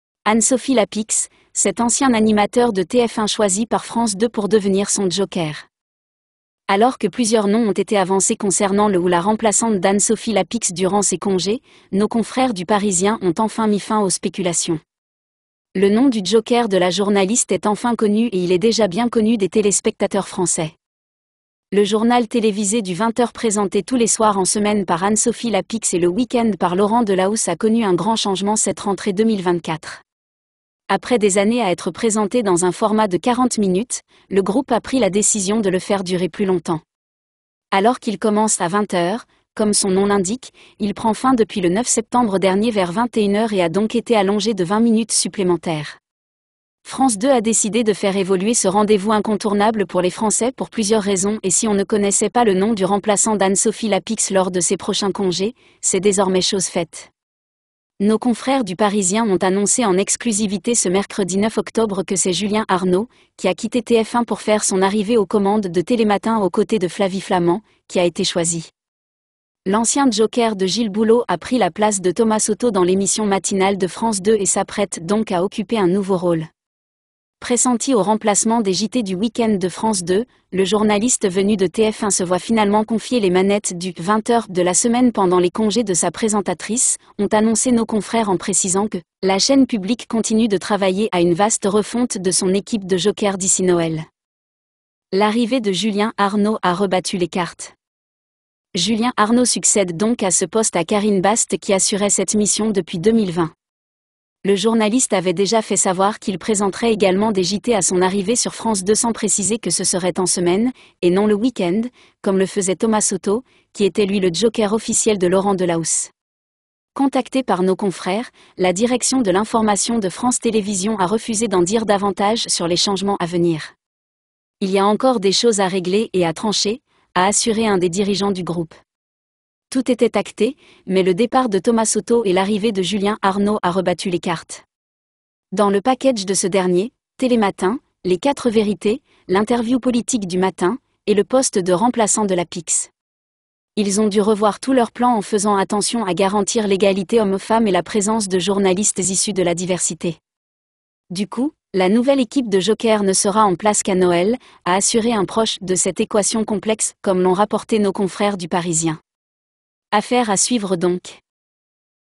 Anne-Sophie Lapix : cet ancien animateur de TF1 choisi par France 2 pour devenir son joker
anne-sophie lapix ne savait pas que la caméra enregistrait toujours